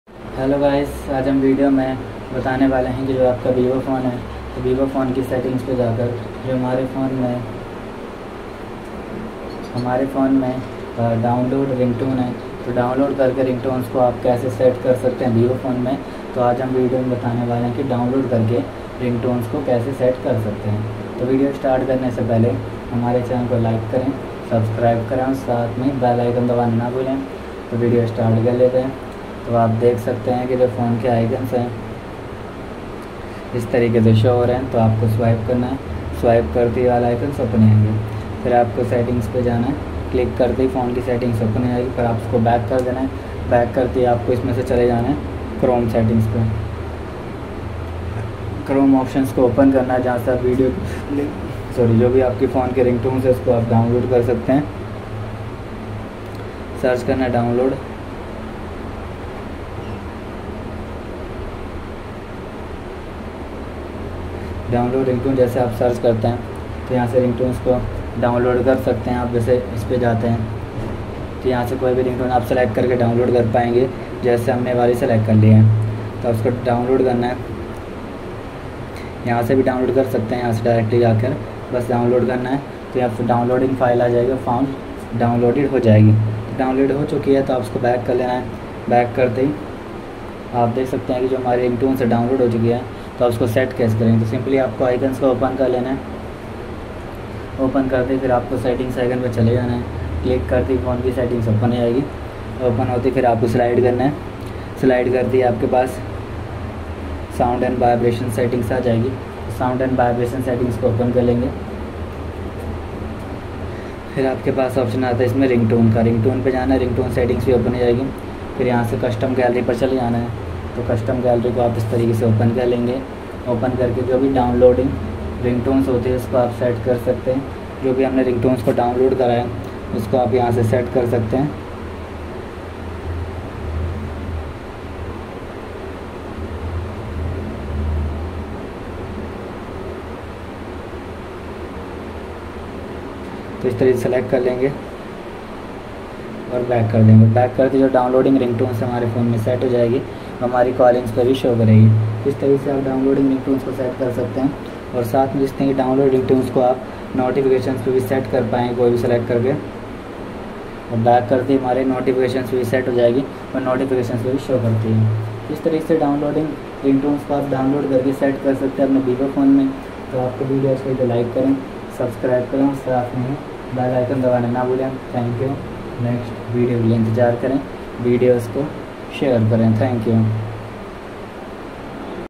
How to download ringtone in Vivo T2 5g, Vivo mobile me ringtone download & set kaise kare
Best powerful CPU under 10k

60-Inch Tripod with Bag

7 Socket 6A Extension Board

multiplug Extension